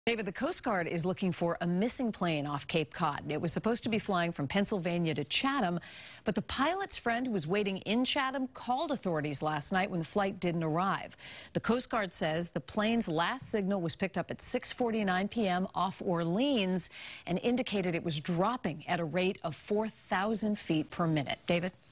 Coast Guard Searching For Missing Plane Off Cape Cod
Search teams were launched late Sunday night.